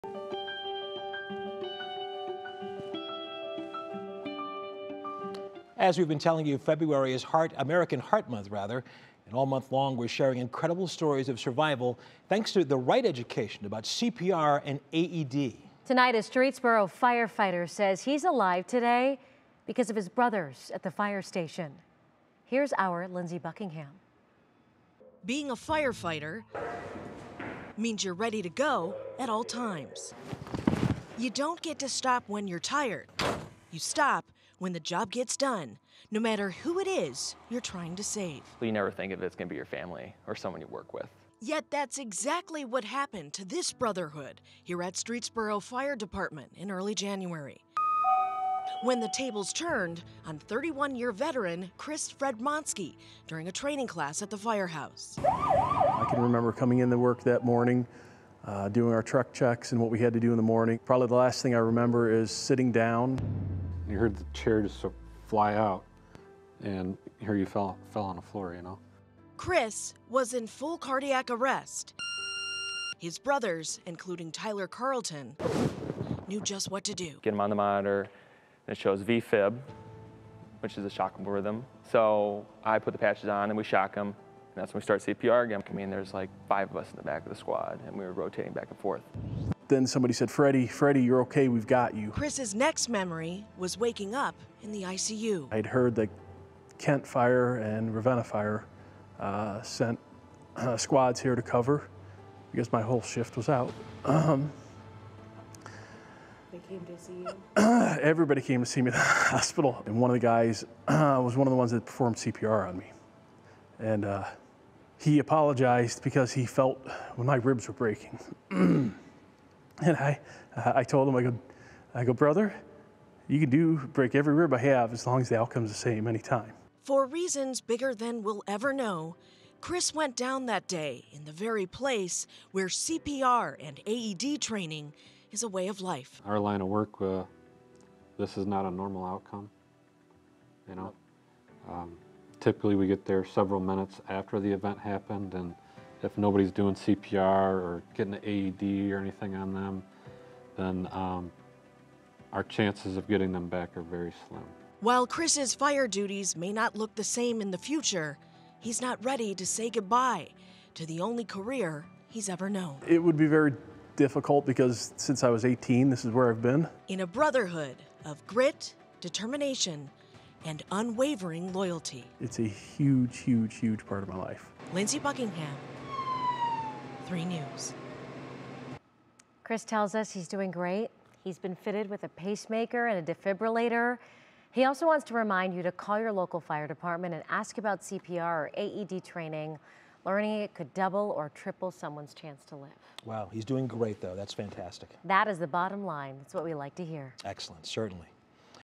Streetsboro firefighter on surviving cardiac arrest: 'The outpouring, it's been really incredible'
Being a firefighter means you're ready to go at all times. You don't get to stop when you're tired, you stop when the work gets done ... no matter who it is you're trying to save.

And, especially, when it's someone you love.

"You never think it's gonna be your family or someone you work with," said first-year Streetsboro firefighter, Tyler Carlton.

Yet, that's exactly what Tyler and his shift buddies encountered at the beginning of January, when 31-year veteran, Chris Fredmonsky, collapsed during a training class at the Streetsboro Fire Department.

"I can remember coming in to work that morning, doing our truck checks and what we had to do in the morning. I knew we had a continuing education class that the doctor was coming in to teach. Probably the last thing I remember is sitting down," Chris told us.